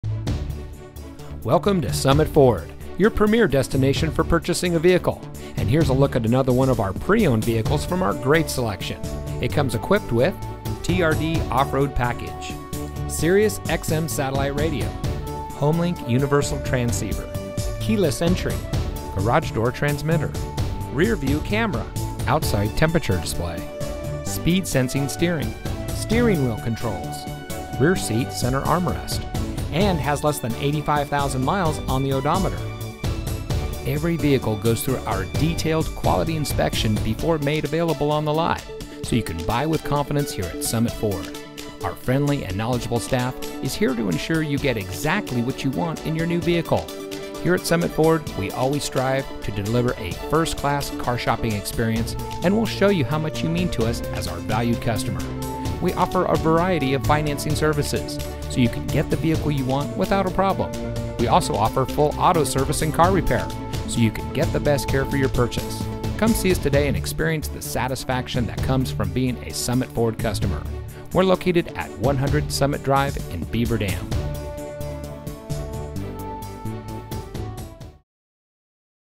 2016 Toyota Tundra CrewMax Used. walk around for sale in Beaver Dam, Wisconsin
2016 Toyota Tundra CrewMax Stock Number: 1526XA Vin:5TFDW5F17GX569068